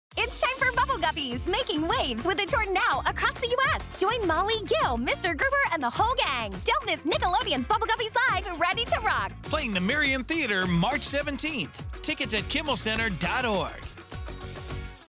Bubble Guppies are coming LIVE to Philadelphia!
The Bubble Guppies are getting ready to rock n’ roll with some totally fin-tastic tunes, so put on your water-wings and jump into a swimsational adventure!

Bubble Guppies Live! Ready to Rock
March 17, 2018 I Merriam Theater